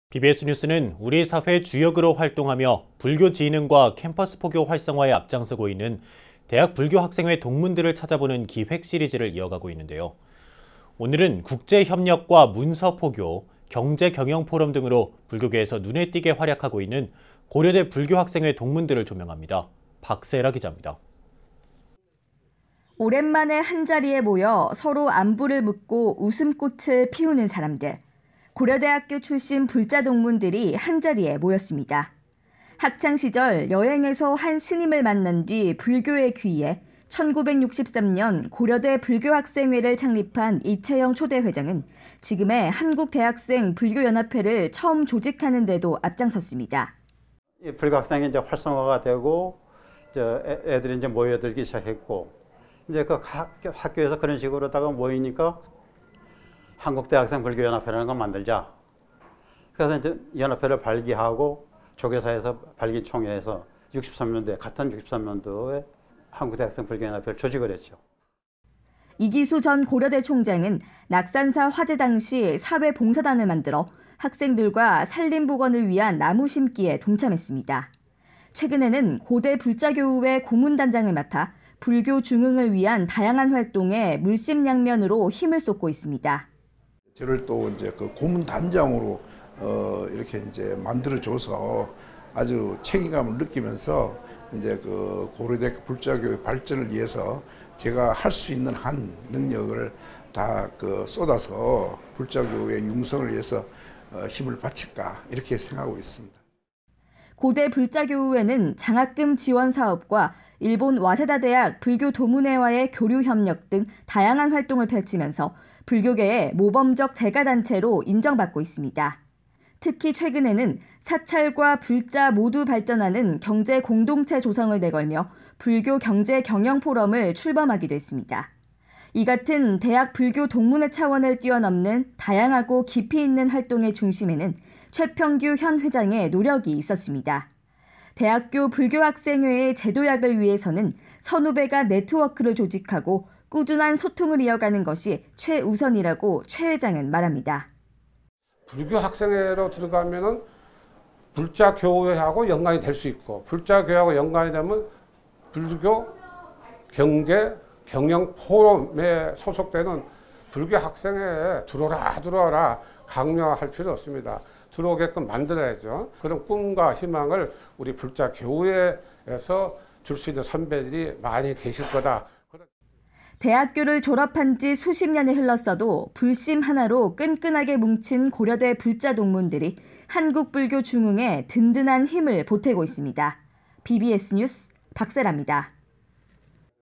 [BBS 뉴스] [기획] '꿈틀거리는 대불련 동문들' ⑧ 고려대
BBS 뉴스는 우리 사회 주역으로 활동하며 불교 진흥과 캠퍼스 포교 활성화에
앞장서고 있는 대학 불교학생회 동문들을 찾아보는 기획 시리즈를 이어가고 있는데요..

오늘은 국제협력과 문서포교, 경제경영포럼 등으로 불교계에서 눈에 띄게 활약하고 있는
고려대 불교학생회 동문들을 조명합니다. 박세라 기자입니다.